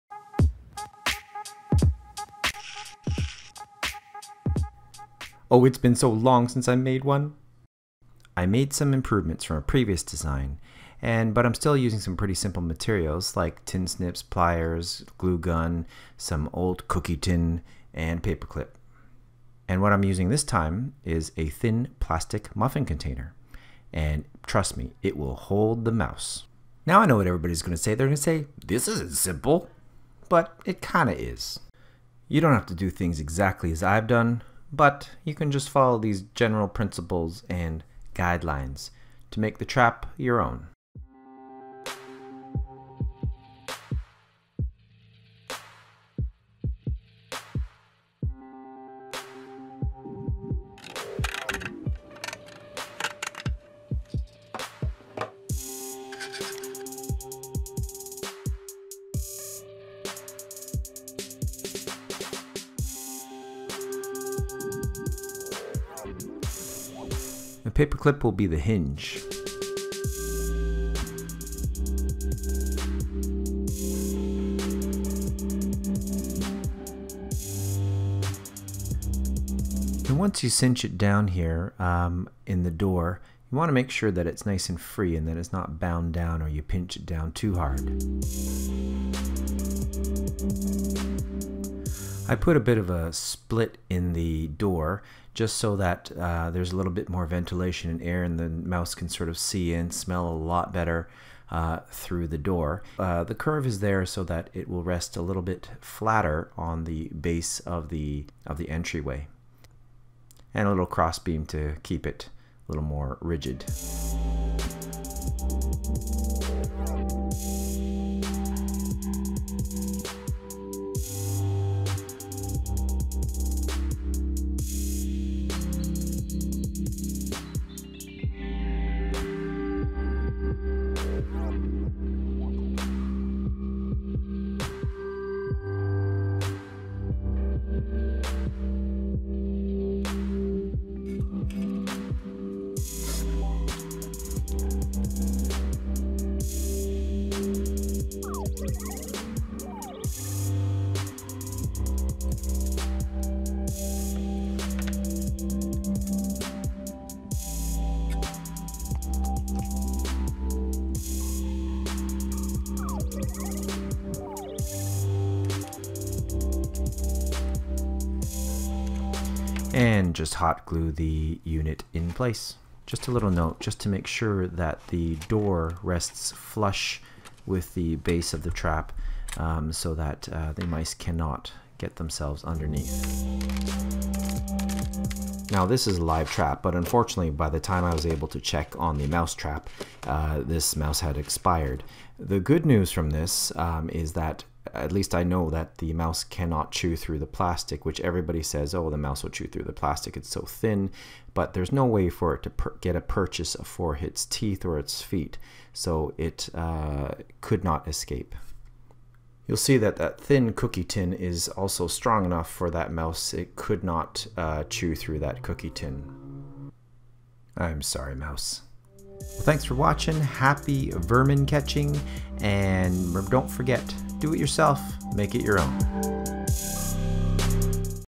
Muffin Mousetrap- A Simple DIY, Live trap!
Materials: muffin container, cookie tin scraps, paper clip, sweat

Yes this is a LIVE trap. No I'm not a lover of vermin.